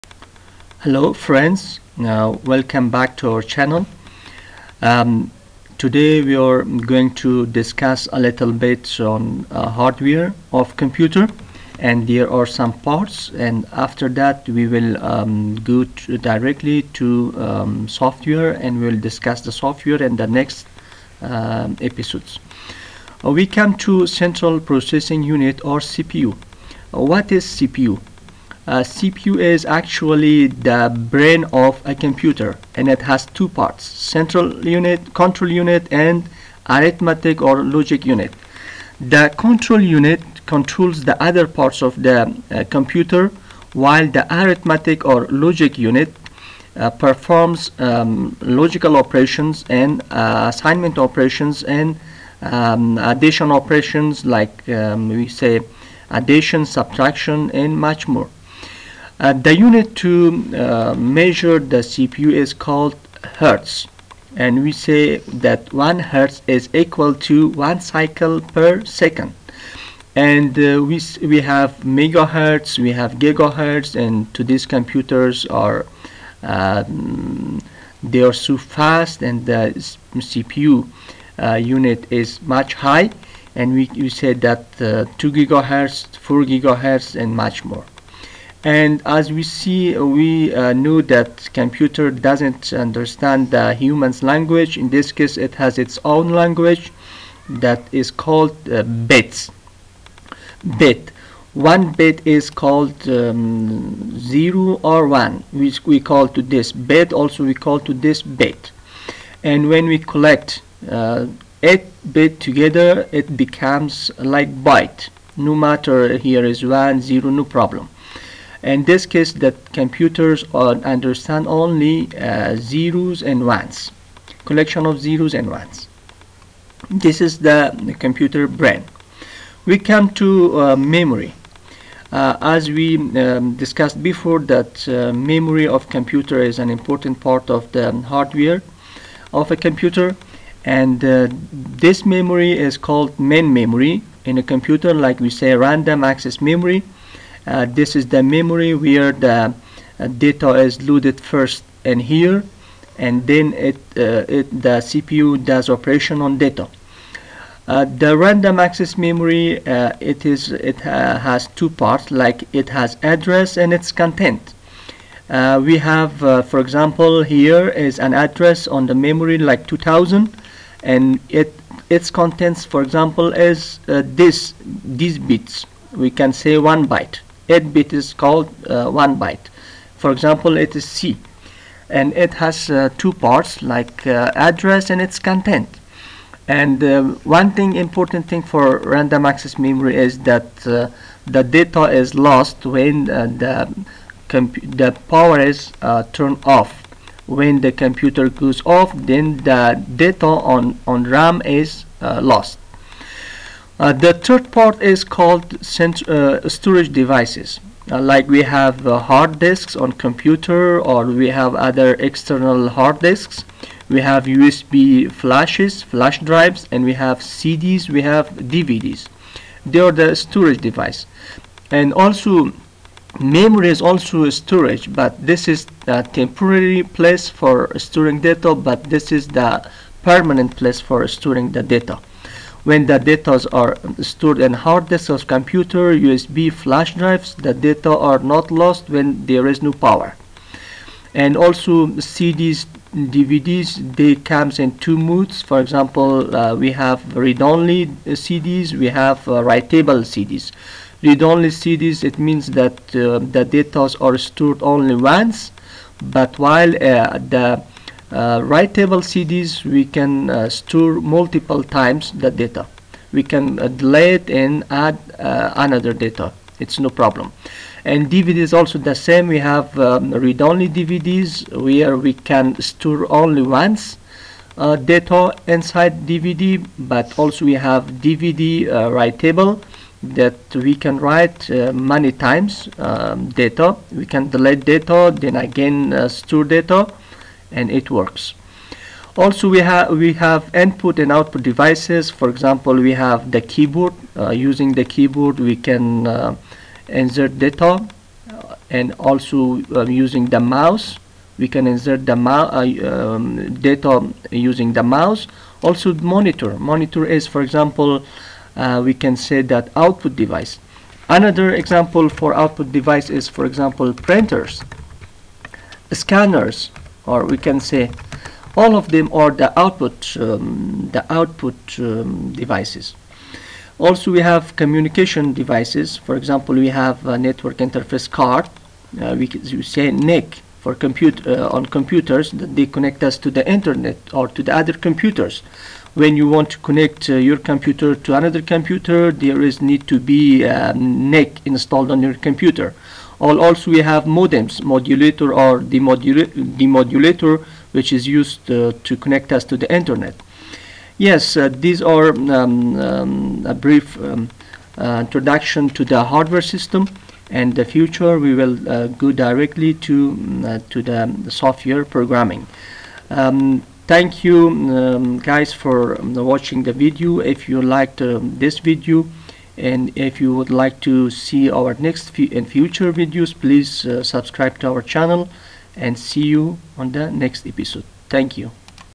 Java Programming tutorial for beginners #2 What is Central Processing Unit, Memory, Hard disk, Input
This videos provides short information about the Hardware of computer and its parts like CPU, RAM, Hard Disk, Input and Output devices and so on.